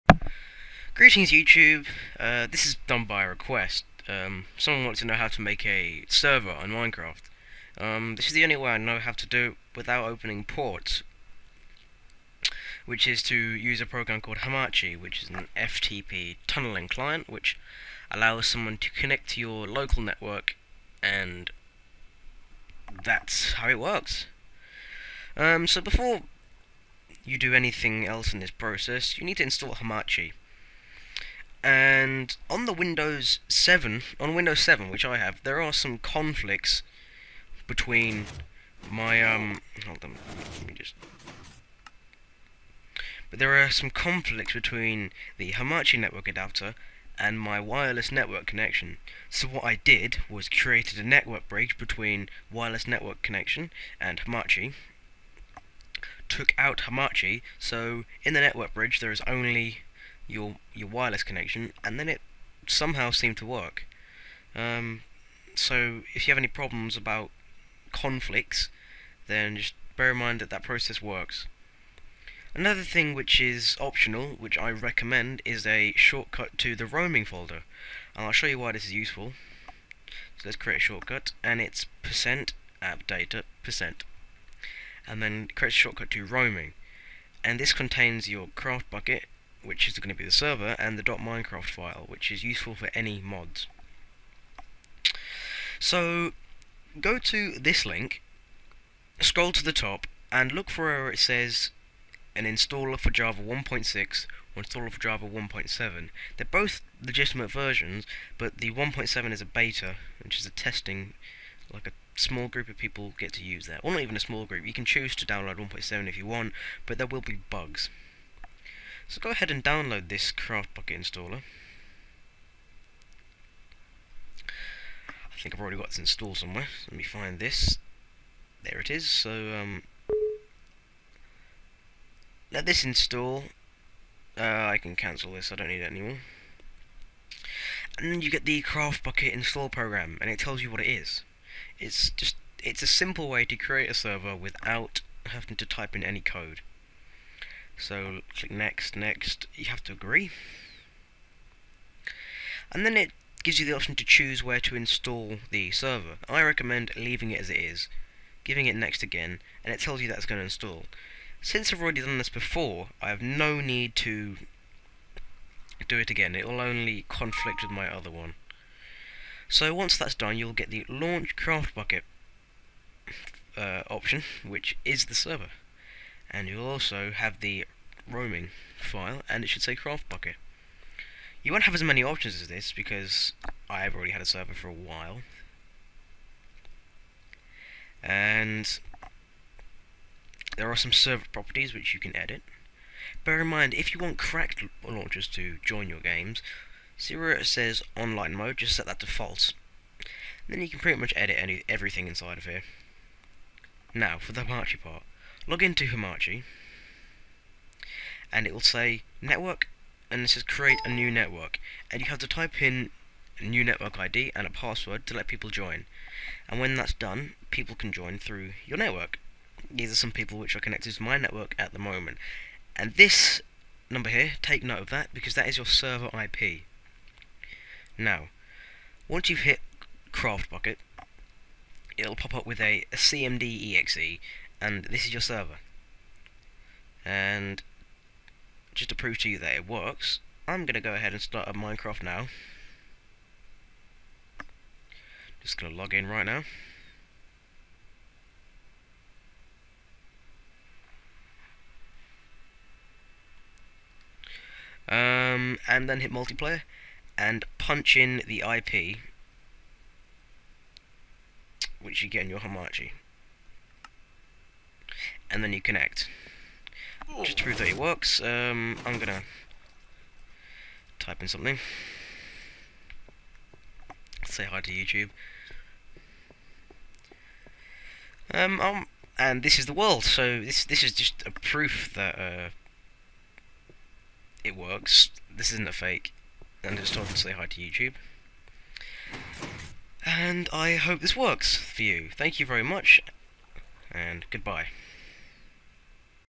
How to make a free Minecraft server 1.0.0
This is the easiest way to create a Minecraft server without adding Ports or messing around with your Net connection. Trust me, it works. If it doesnt for you, you must be doing something wrong.

You will need:
1.) Hamachi - I can't stress how important Hamachi is to this process, as long as you want to remain a free server, it is needed:
- It's an .msi file, don't worry, this is correct.

2.) CraftBukkit - If you don't want to code then this is the tool for you, just let it install.